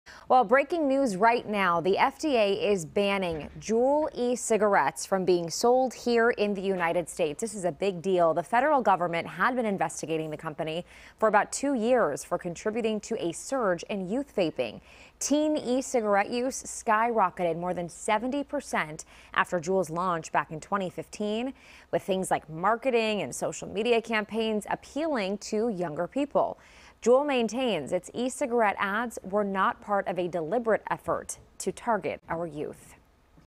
FDA Orders Juul Labs To Remove Products From US Market
The US Food and Drug Administration ordered Juul products removed from the US market as the agency issued marketing denial orders for its vaping devices and pods. Juul devices and four types of pods -- tobacco and menthol-flavored -- cannot be sold or distributed, the FDA said.